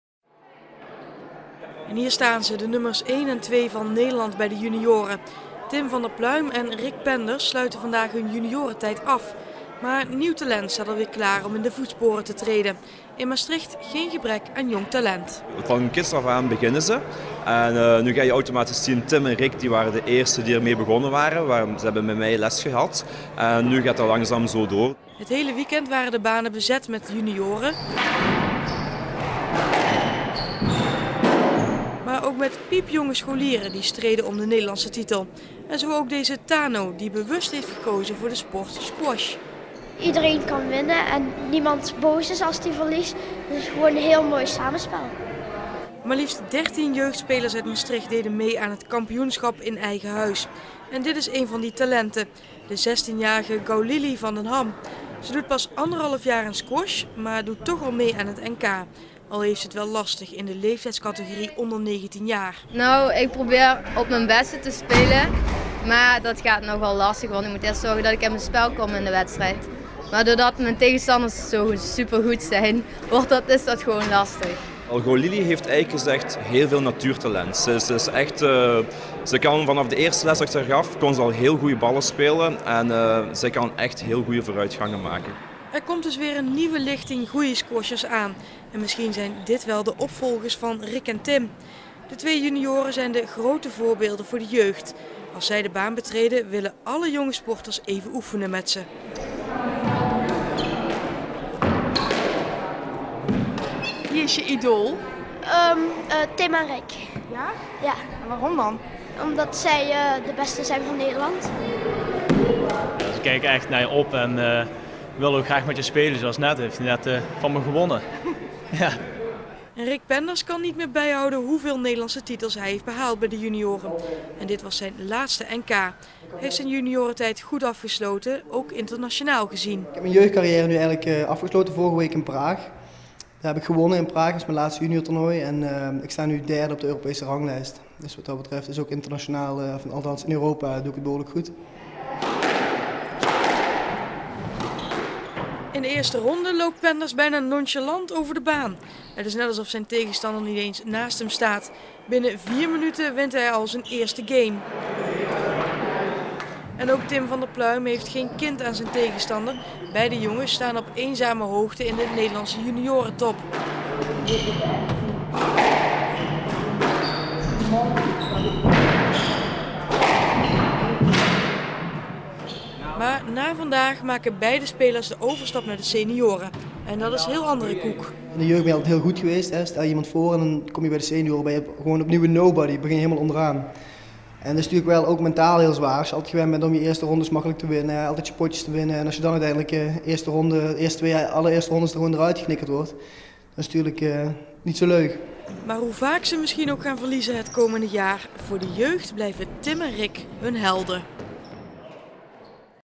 NK jeugdsquash 30-11-2010
Afgelopen weekend vond het NK Squash t/m 19 jaar plaats hier in Maastricht.

Finale ging tussen de 2 Maastrichtse talenten Rik en Tim, beiden door NLcom gesponsord.

Hier de reportage van L1, met veel nlcom in beeld, op baan 1 en de mooie tenues van de heren ..

Vanaf nu gaan beiden heren de strijd aan met de echte mannen en verlaten ze de jeugd.

Over 2 weken is de echte NK Squash in Amsterdam, we zijn benieuwd hoever ze daar kunnen komen.